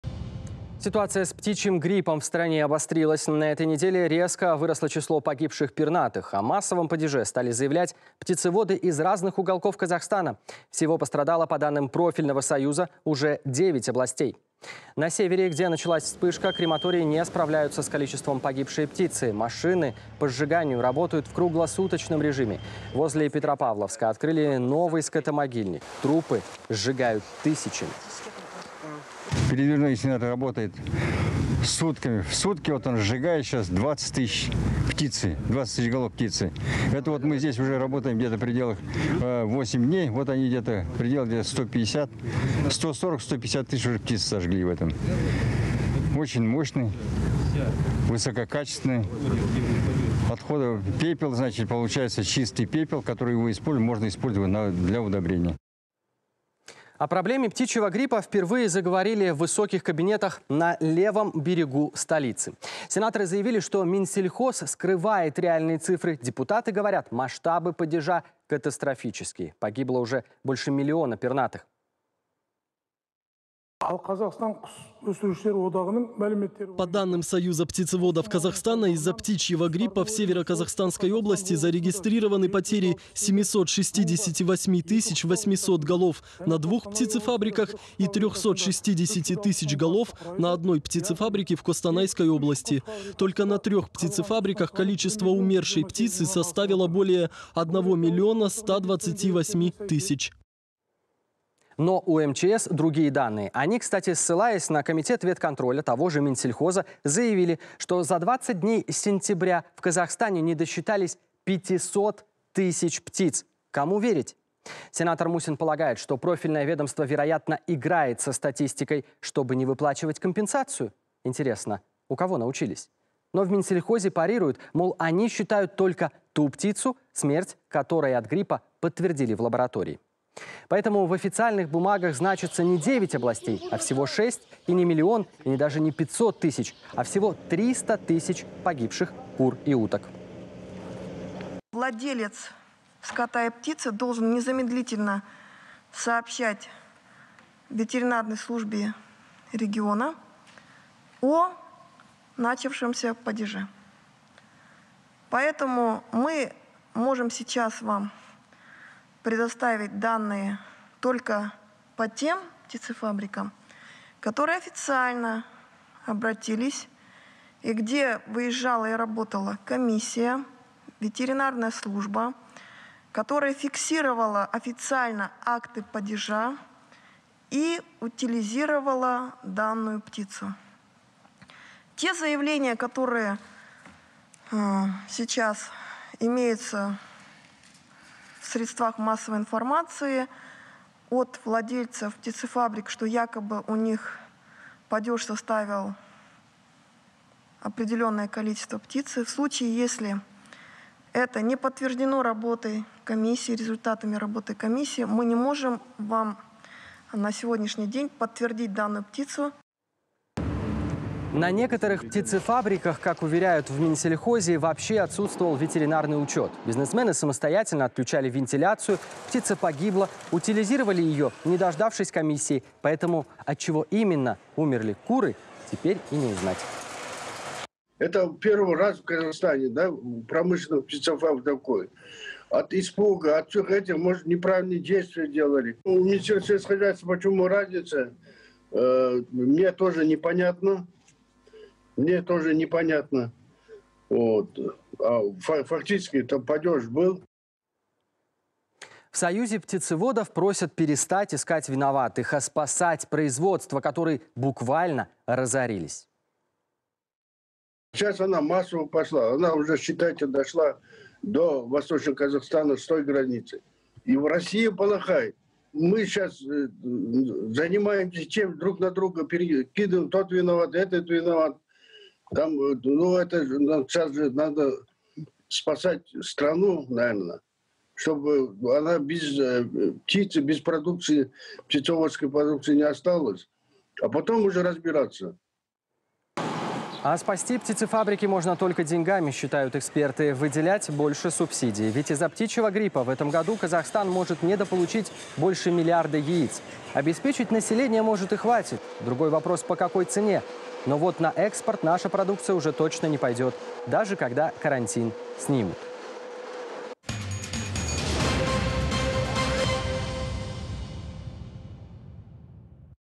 Из-за птичьего гриппа Казахстан недополучит миллиард яиц
Ситуация с птичьим гриппом в стране обострилась. На этой неделе резко выросло число погибших пернатых. О массовом падеже стали заявлять птицеводы из разных уголков Казахстана. Всего пострадало, по данным профильного союза, уже девять областей.